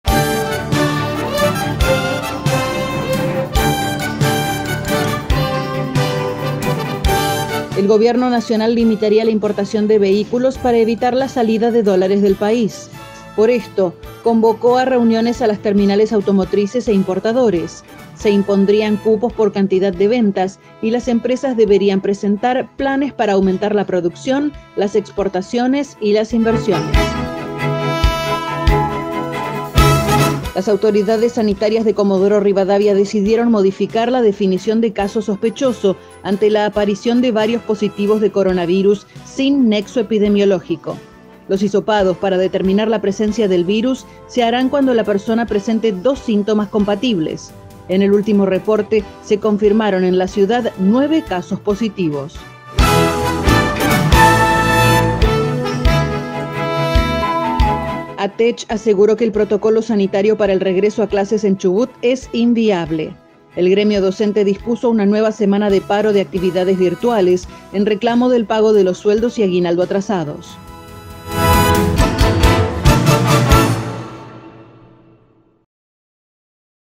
Breves- 18 de agosto 2020.
1.- Limitarían importaciones.
2.- Criterio de hisopado.
3.- Protocolo inviable.